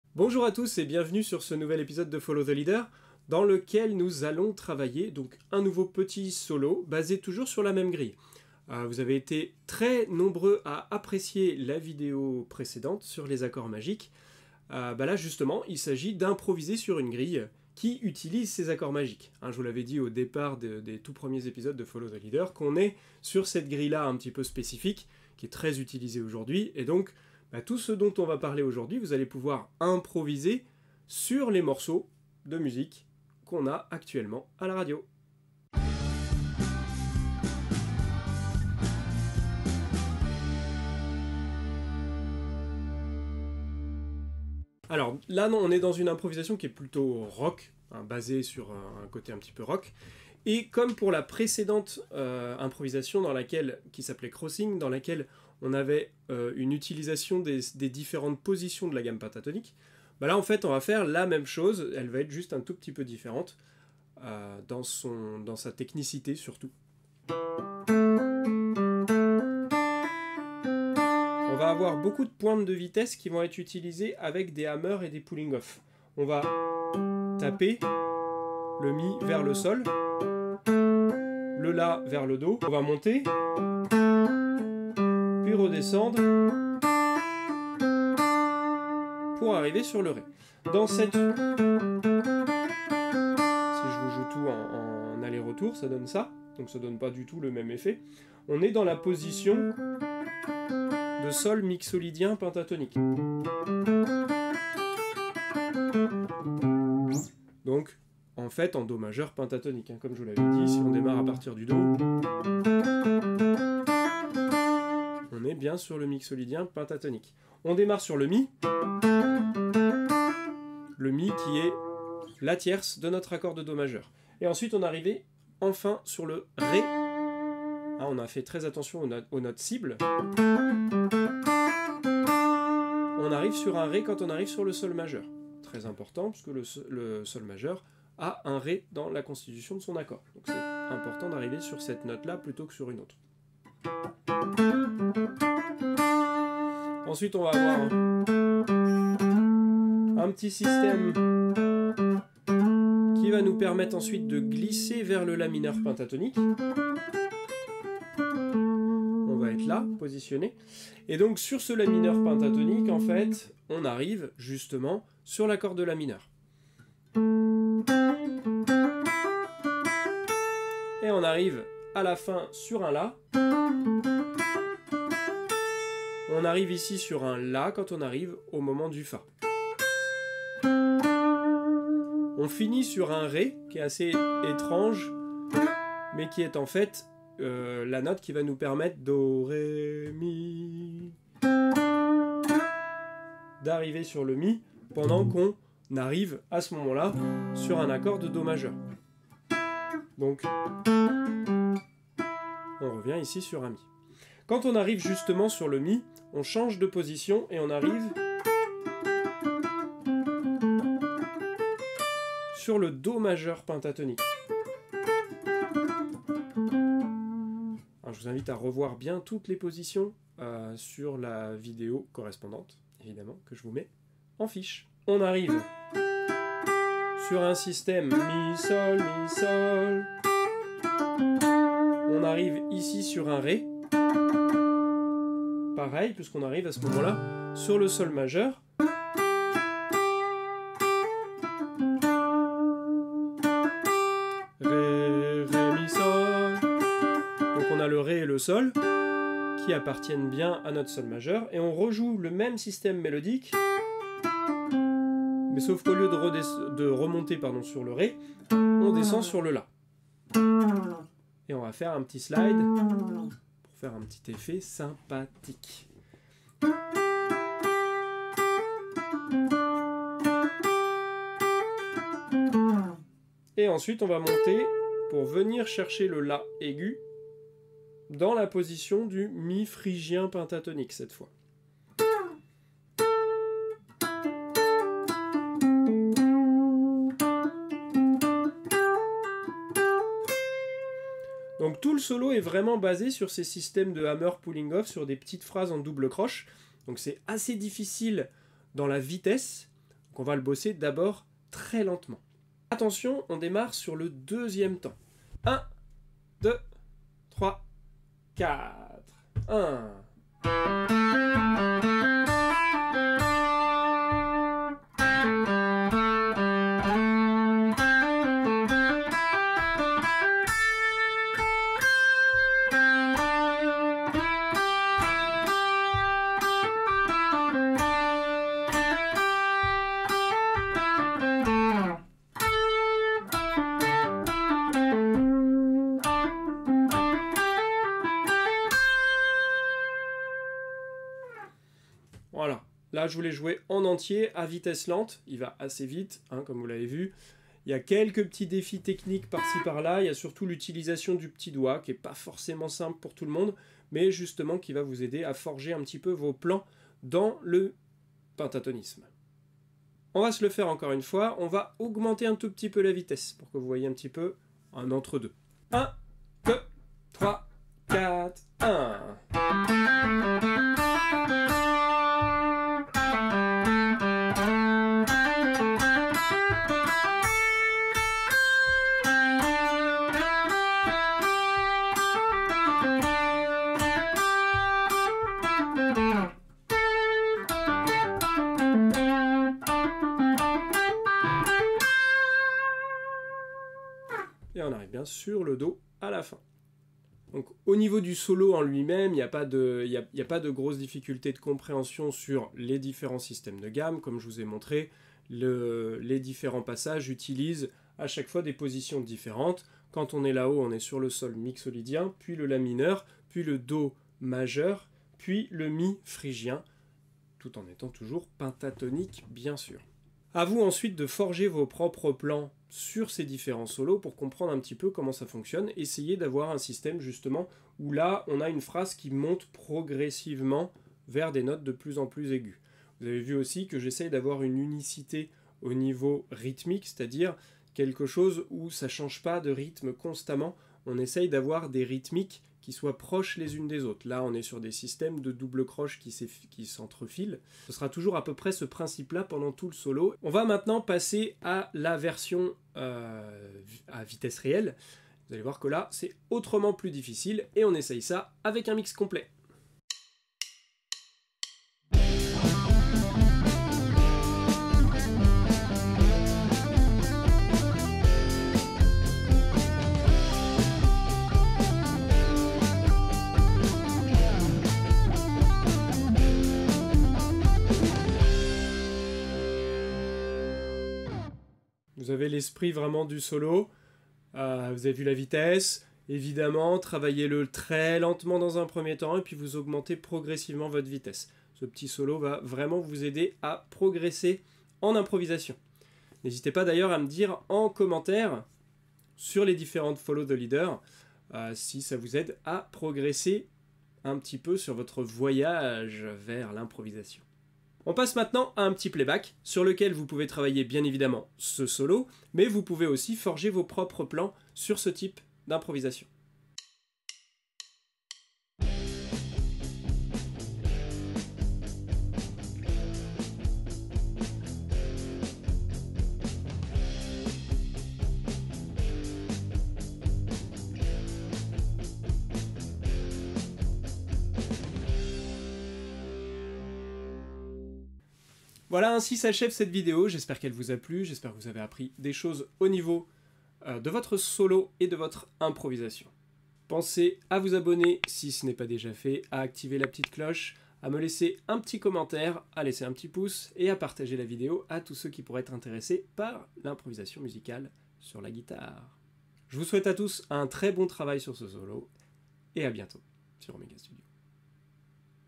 comment improviser avec les gammes pentatoniques : croisement sur Crossing#2 (Guitare)
Bienvenue et Merci à tous les nouveaux, la vidéo précédente semble vous avoir plu ! Du coup, je vous propose cette semaine de travailler l'improvisation guitare sur la grille des accords magiques. On travaille sur toutes les positions de la gamme pentatonique de Do Maj !
Vous retrouverez un mix complet et un playback pour vous exercer ! N'hésitez pas à me dire en commentaire si vous progressez sur l'improvisation !

Micro Audio-Technica AT2020 USB
Focusrite 4i4
Webcam Logitech C920
Appareil Photo Nikon d5200

Timeline :
00:00 Intro
00:35 Générique
00:45 Explication de gammes utilisées
01:07 Travail de la première phrase (Mixo)
02:36 Travail de la deuxième phrase (mineur)
03:19 Travail de la troisième phrase (Majeur)
04:32 Travail de la dernière phrase (phrygien)
05:00 Travail à vitesse lente du solo complet
06:00 Travail à vitesse légèrement plus vite
06:36 Construction du solo
07:30 Mix Complet vitesse réelle
08:02 Derniers Conseils
08:28 Playback
08:59 End